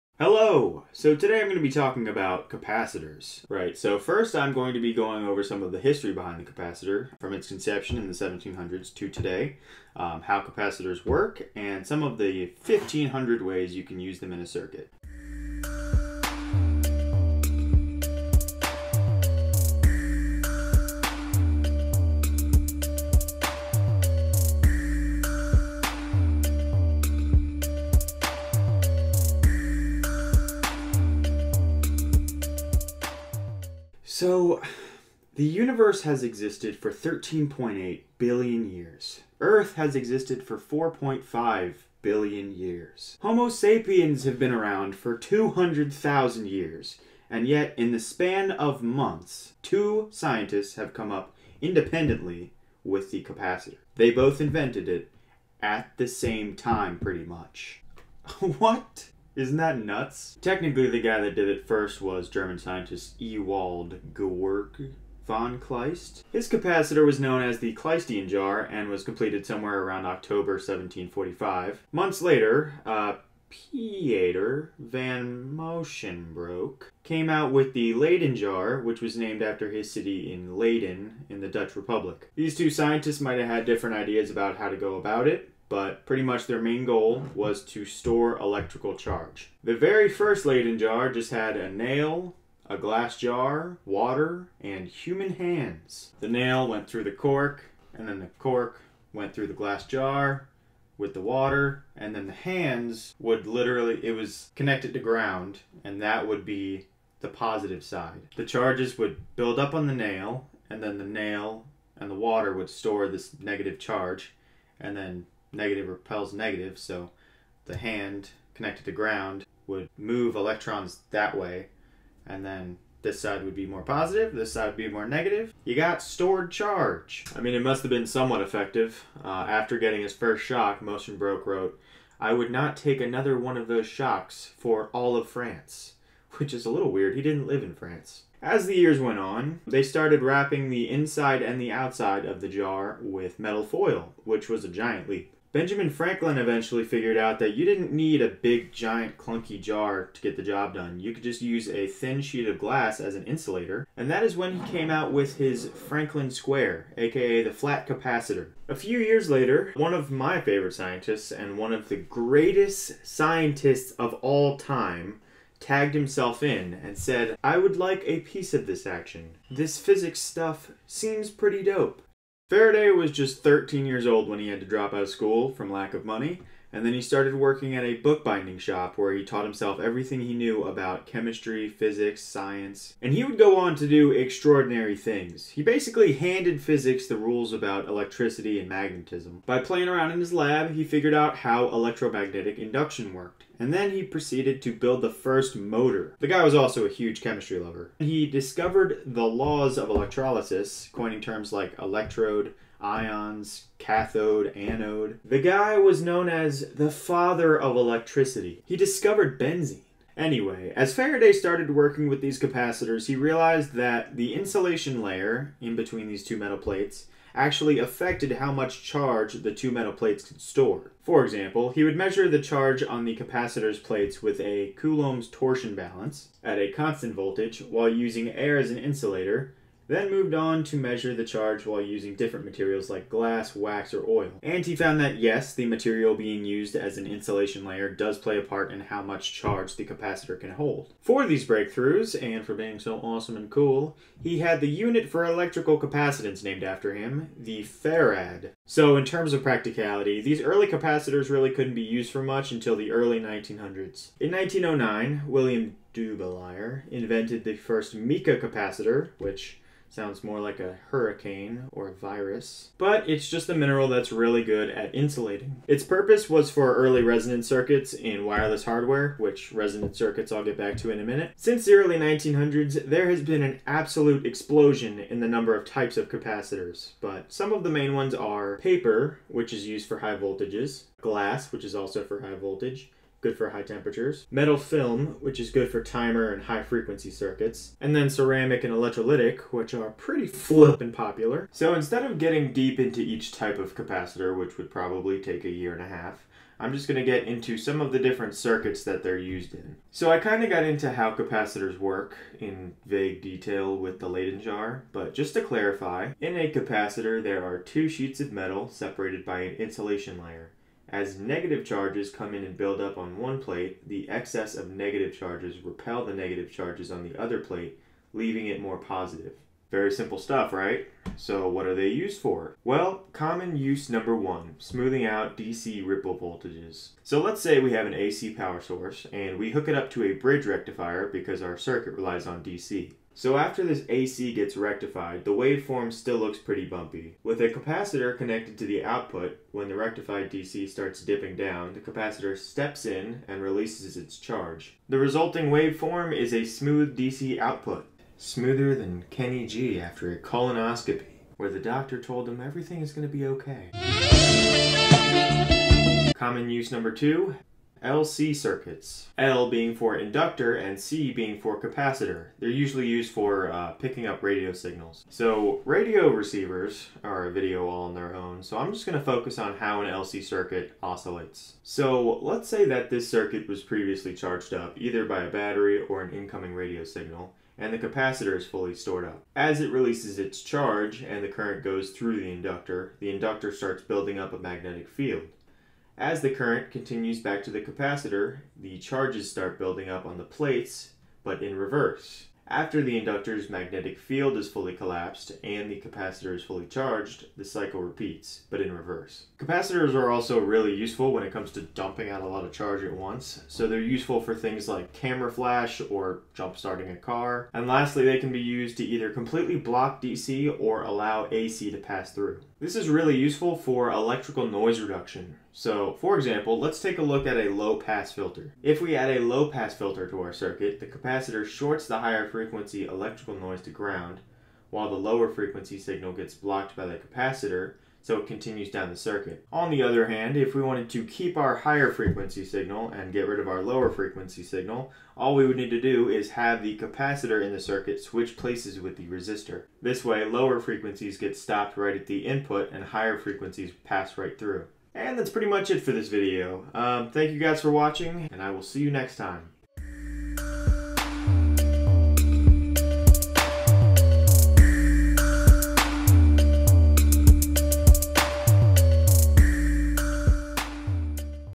How capacitors work, and how they're used in circuits today
In this tutorial, I get into some of the history behind the capacitor,  as well as how capacitors work internally, and how to use them in a circuit.